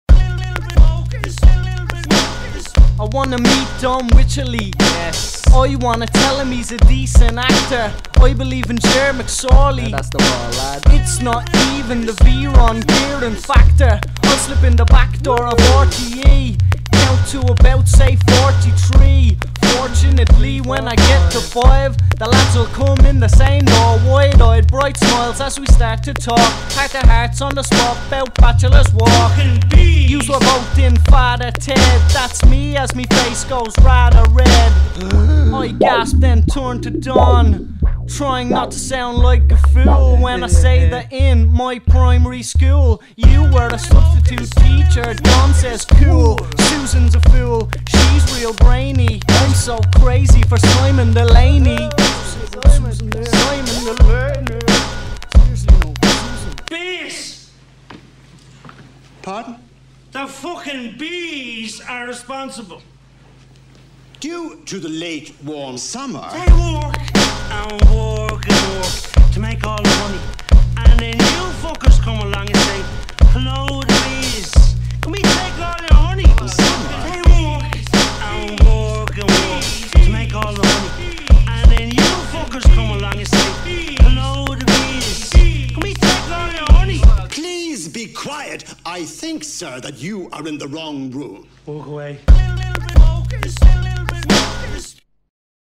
Simon Delaney
simon delaney, don wycherley & keith mcerklean having a laugh with mo-t.